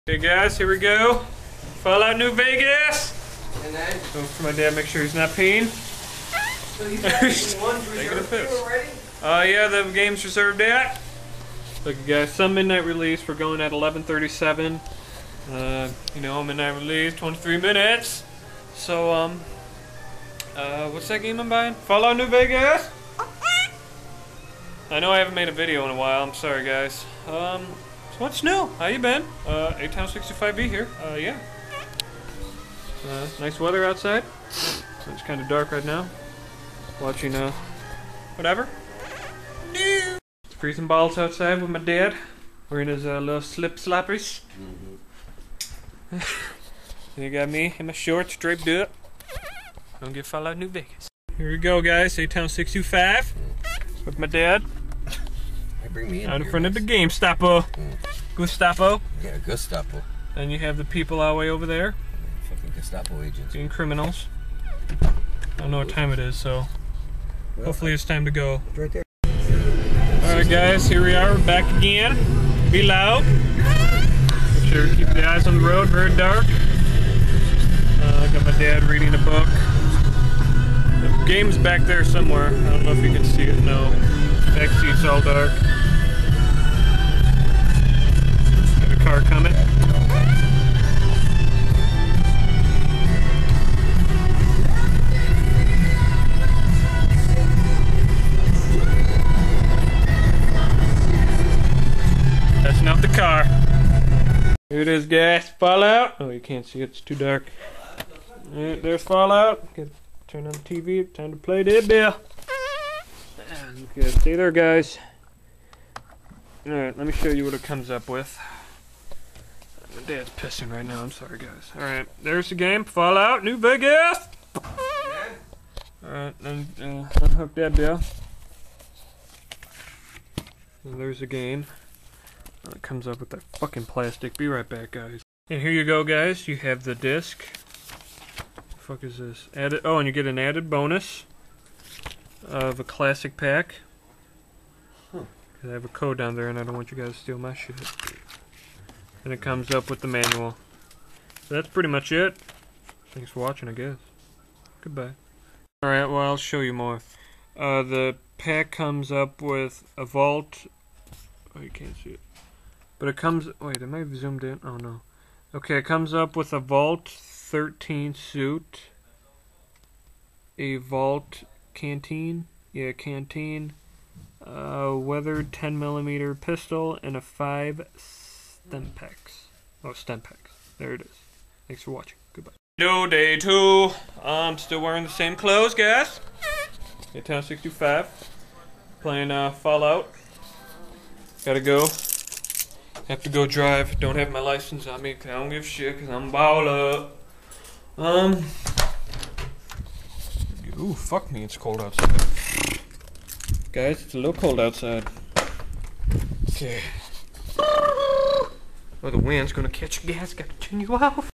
Midnight Release New Vegas
My dad and I going to gamestop getting vegas.